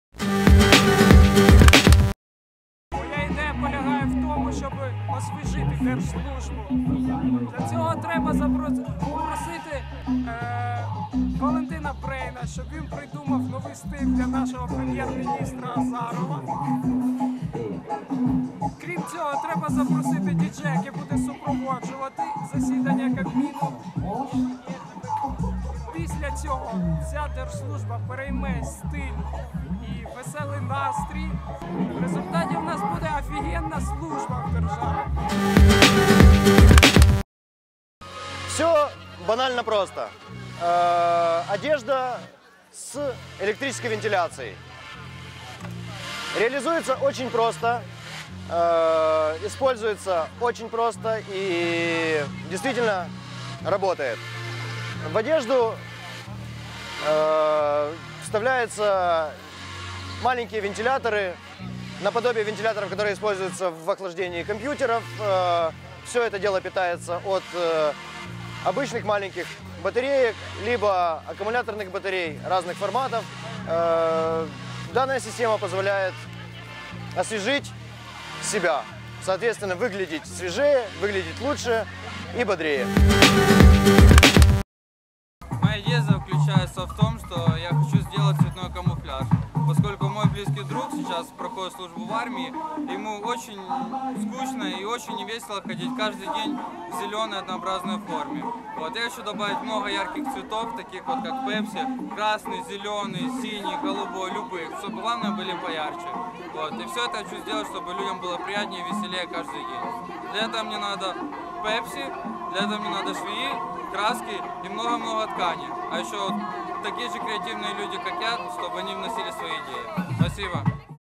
Ідеї випробування "Скажи своє слово про стиль" part 4.
Ідеї гостей вечірки Pepsi Refresh Market, які стали учасниками випробування "Скажи своє слово про стиль"